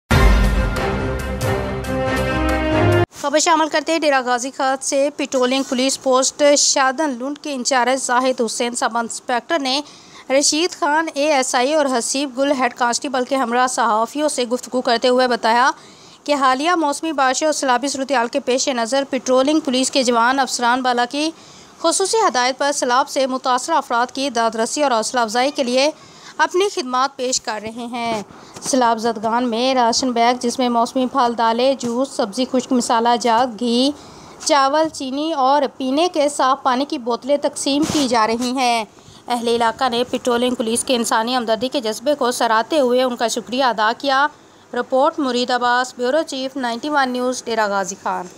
حالیہ موسمی بارشوں اور سیلابی صورتحال کے پیش نظر پٹرولنگ پولیس کے جوان
خبرشامل کرتے ہیں ڈیرہ غازی خان سے
پیٹرولنگ پولیس پوسٹ شادن لنڈ کے انچارج زاہد حسین سب انسپکٹر نے رشیدخان اے ایس آئی اور حسیب گل ہیڈ کانسٹیبل کے ہمراہ صحافیوں سے گفتگو کرتے ہوئے بتایا کہ حالیہ موسمی بارشوں اور سیلابی صورتحال کے پیش نظر پٹرولنگ پولیس کے جوان، افسران بالا کی خصوصی ہدایت پر سلاب سے متاثرہ افراد کی داد رسی اور حوصلہ افزائی کے لئے اپنی خدمات پیش کر رہے ہیں سیلاب زدگان میں راشن بیگ جس میں موسمی پھل دالیں جوس سبزی خشک مصالحہ جات گھی چاول چینی اور پینے کے صاف پانی کی بوتلیں تقسیم کی جا رہی ہیں
 اہل علاقہ نے پٹرولنگ پولیس کے انسانی ہمدردی کے جذبے کو سراہتے ہوئے انکا شکریہ ادا کیا
رپورٹ مرید عباس بیوروچیف91نیوز ڈیرہ غازی خان